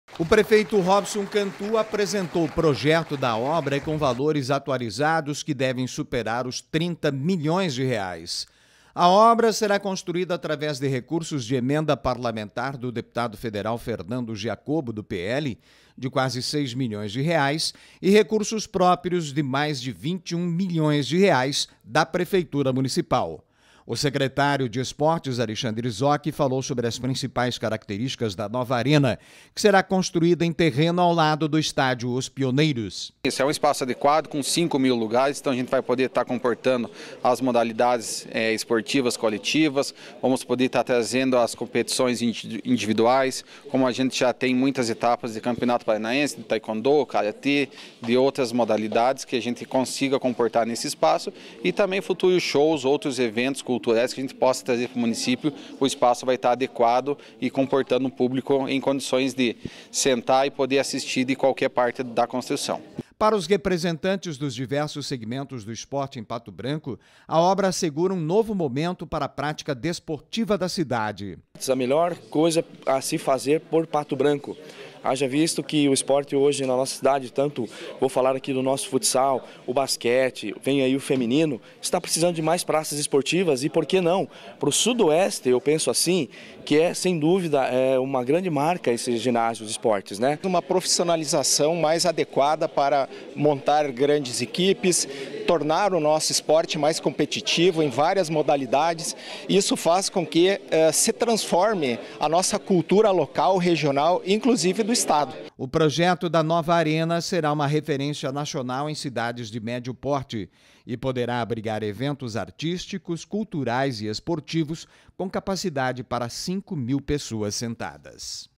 Lançado processo de licitação da nova Arena de Pato Branco
Gestão Pública: Lançado o edital de licitação para a construção da nova Arena do município.